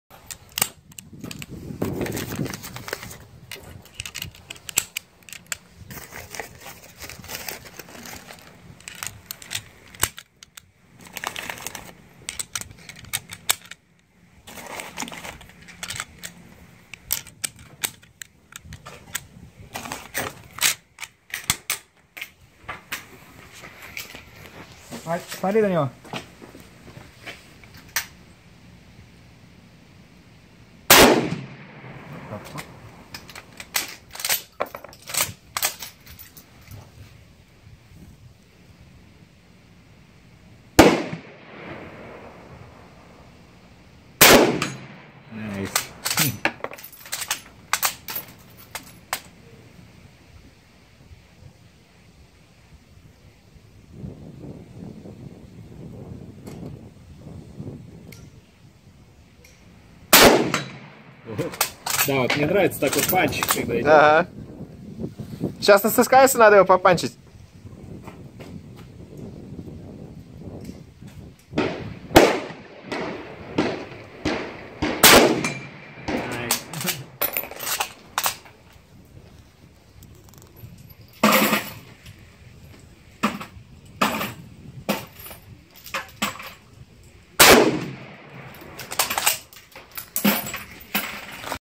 Mosin–Nagant 91/30 Part 35
Mosin–Nagant 91/30 was the standard issue weapon of Soviet troops and millions of the rifles were produced and used in World War II by the largest mobilized army in history